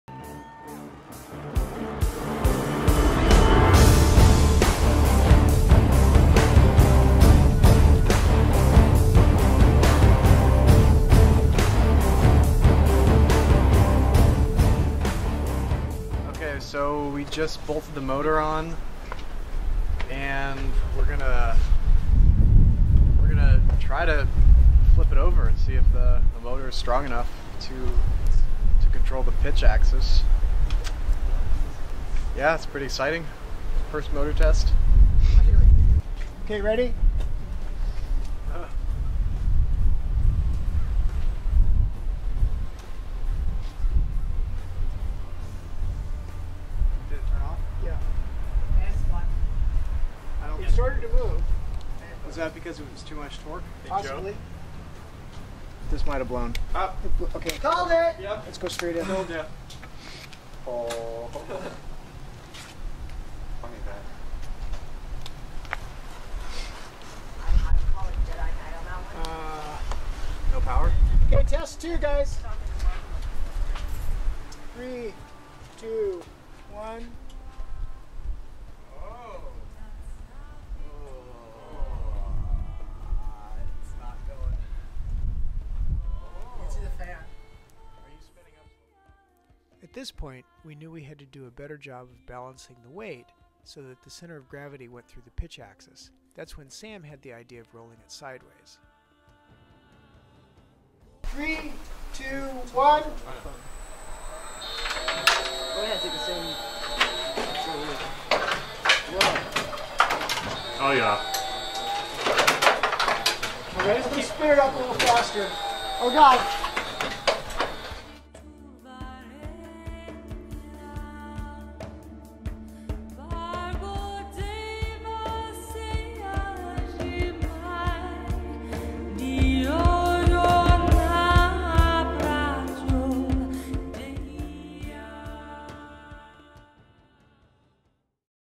The Viper - Week 22 - Pitch motor
Today was a big milestone. This was our first test of motor control of the motion platform. We've been working toward this day for months, and by the end of the day we had a complete success, though it didn't appear that way on the initial trial.